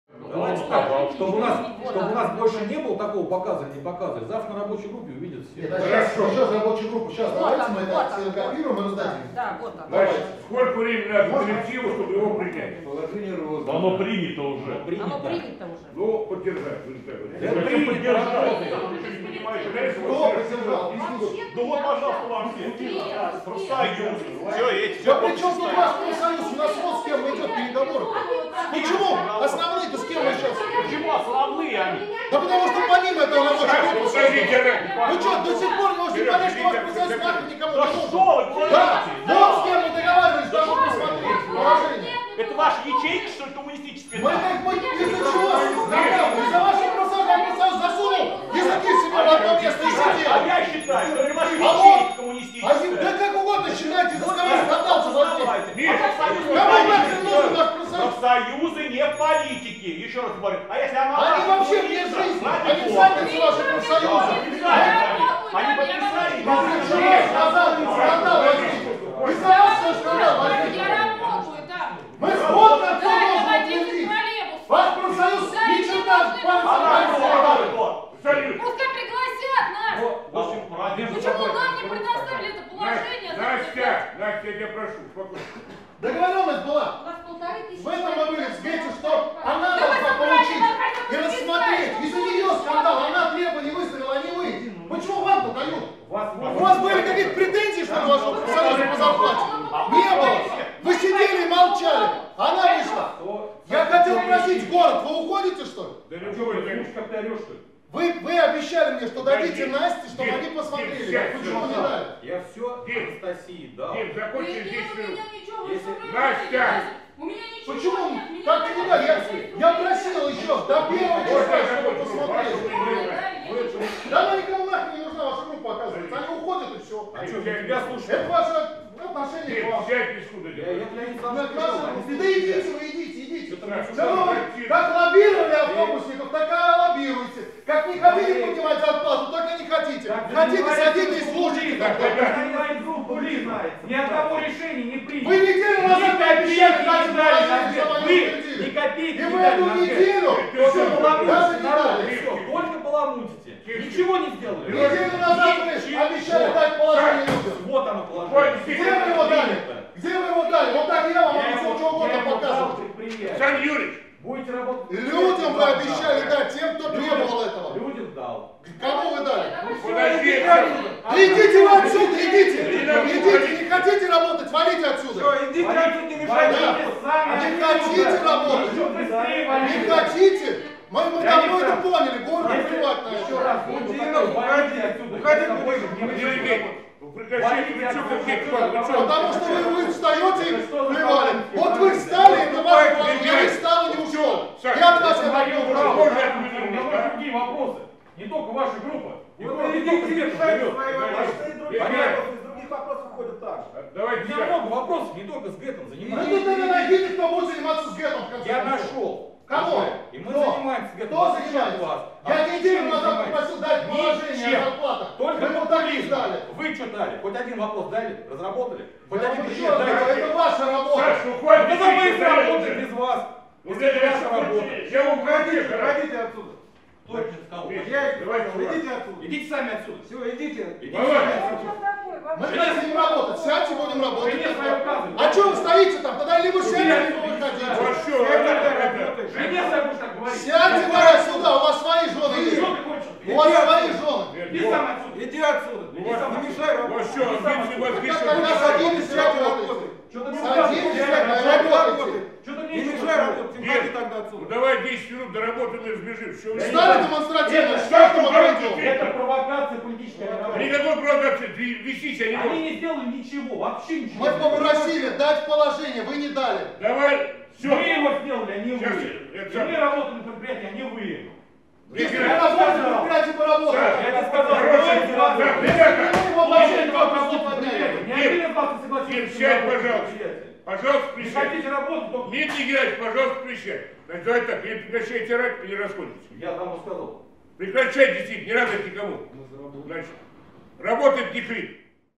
Кризис в "СГЭТ". Обсуждение в облдуме закончилось перепалкой депутатов
Очередной скандал произошел в стенах Саратовской облдумы. На этот раз страсти до предела накалились во время заседания рабочей группы по проблемам в МУПП "Саратовгорэлектротранс". В роли непримиримых оппонентов выступили депутат Александр Анидалов (КПРФ) и депутат гордумы Дмитрий Кудинов.

Сначала коммунист обвинил официальный профсоюз СГЭТ в том, что они "ничего не делают", а потом раскритиковал своего городского коллегу.

По мнению оратора, Дмитрий Кудинов причастен к лоббированию автобусных предприятий. Подробности на сайте ИА "Взгляд-инфо".